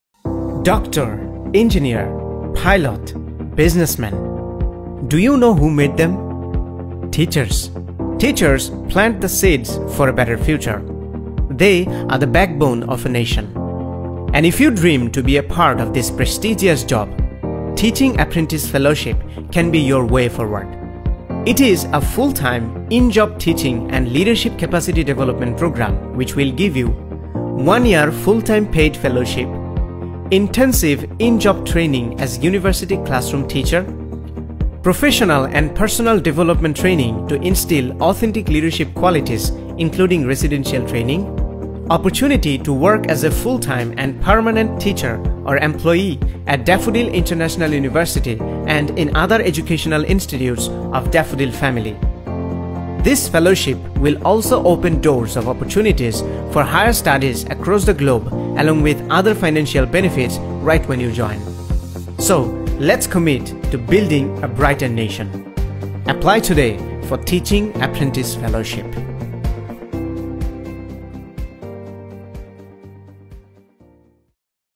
Start Career as University Teacher, Join Teaching Apprentice Fellowship
Start Career as University Teacher
Join Teaching Apprentice Fellowship
:: Be a true leader to transform lives ::
💢Teaching Apprentice Fellowship (TAF) is an educational leadership development program. The demand for excellence in education, especially in higher education, is a major concern today. Knowledgeable and competent faculty members are one of the key priorities to achieve desired excellence. This is the place where TAF will help candidates to earn the required competencies, experiences and confidence to serve as a teacher to create excellence in education and a better sustainable future of Bangladesh.
💢Benefits
👉TAF fellows will have different teaching positions such as TAF TA (Teaching Assistant), TAF RA (Research Associate)
👉A competitive monthly salary for the fellowship at a range of BDT 10,000 to 50,000 (Salary will be unique for each fellow based on their experience, commitment &  performance) TAF TA : BDT 10,000 – 20,000  TAF RA: BDT 20,000 - 50,000
👉TAF Fellows will get opportunities for higher education in DIU & in partner universities around the globe.
👉TAF Fellows will get opportunities for Ph.D. and Research funding in home and abroad.
👉Professional & Personal Development training to instill authentic leadership qualities
👉Opportunity to work as a full time & permanent teacher/employee at Daffodil International University
👉TAF fellows can apply for teaching position in different universities and educational institutions
💢Eligibility (Who can Apply)
👉Graduates/Masters of local and foreign universities can apply.
 👉Minimum GPA: SSC 4.0, HSC 4.0, Bachelor's CGPA 3.50 for Faculty of Science & Information Technology, Faculty of Engineering, Faculty of Business & Entrepreneurship, Faculty of Allied Health Sciences and CGPA 3.00 for Faculty of Humanities & social Sciences
👉(O’ level CGPA 3.5 (minimum 3 Bs and 2Cs) & CGPA 3.0 at A’ Level, Minimum 2 subjects, no ‘D’ will be allowed)
 👉Applicants with IELTS band score 7.5 or above will have preference
 👉Applicants with research publication will have preference
👉Applicants with industry experiences will have preference
✅Application Open : November 11
✅Application Deadline: November 28
💢Departments:
👉Business Administration
👉Business Studies
👉Real Estate
👉Tourism & Hospitality Management
👉Innovation & Entrepreneurship
👉Computer Science & Engineering
👉Computing & Information System (CIS)
👉Software Engineering
👉Environmental Science and Disaster Management
👉Multimedia & Creative Technology (MCT)
👉General Educational Development (GED)
👉Information Technology and Management
👉Information & Communication Engineering
👉Textile Engineering
👉Electrical & Electronic Engineering
👉Architecture
👉Civil Engineering
👉Pharmacy
👉Public Health
👉Nutrition & Food Engineering
👉English
👉Law
👉Journalism & Mass Communication
👉Development Studies
👉Information Science and Library Management.